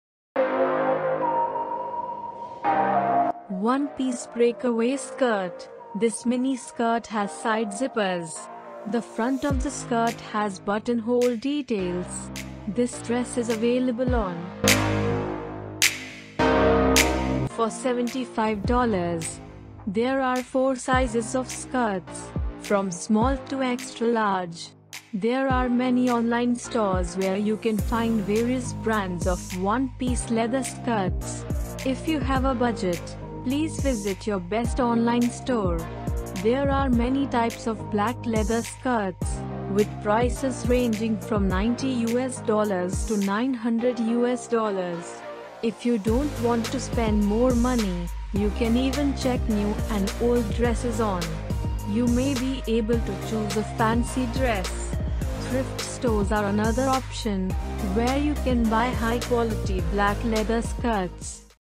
Curvy pleated skirts, Choose economical best match skirts, Plus size Try On Haul Skirts #54
one piece Break Away skirt: This mini skirt has side zippers. The front of the skirt has buttonhole details. There are four sizes of skirts, from small to extra-large.